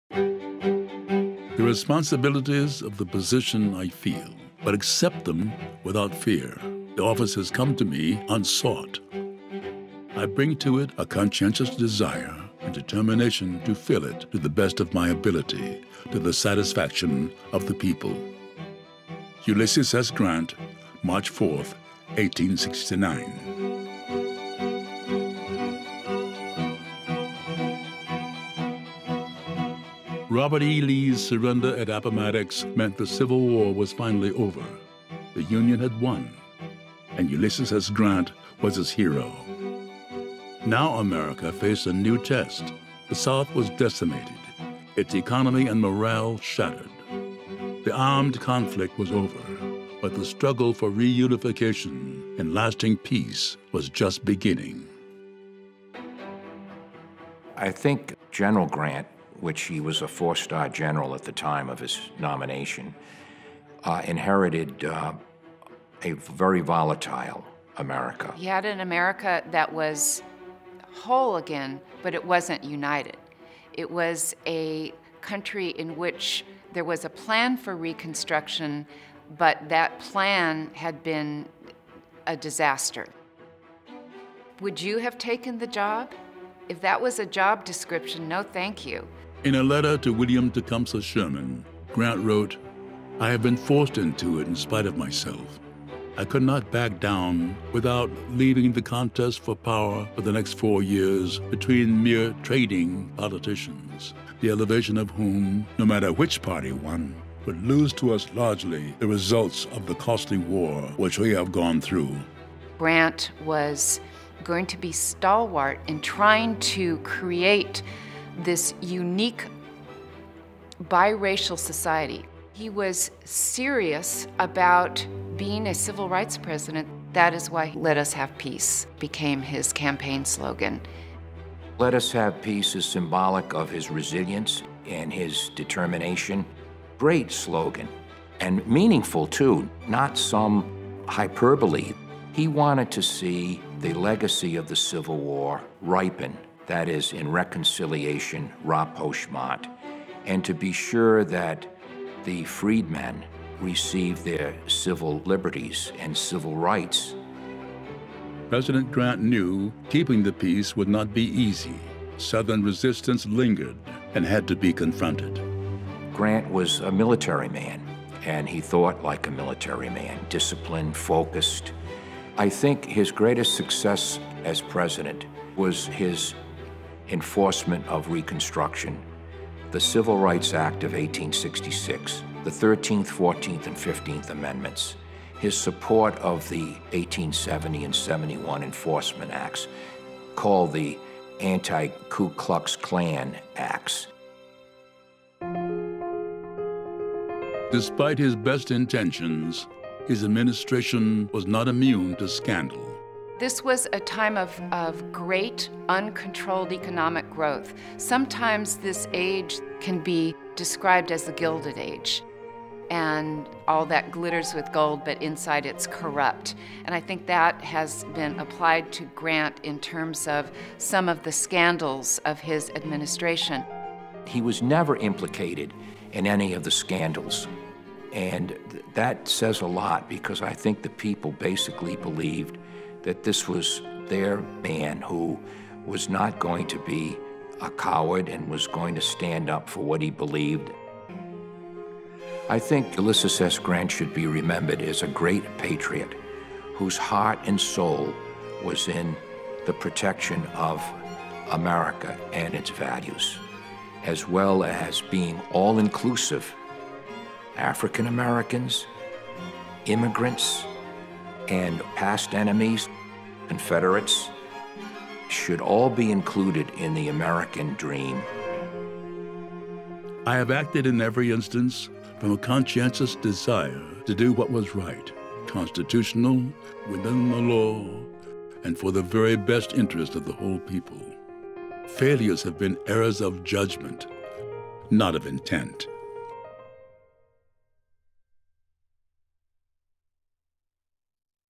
Ulysses S. Grant: President
This short documentary on Ulysses S. Grant as a President was created for the U. S. Grant Presidential Library Museum.

“Let us have peace”: Consider the facts of Grant’s presidency in this video and examine the daunting challenges that Grant faced in trying to reunite the country after four years of bitter war.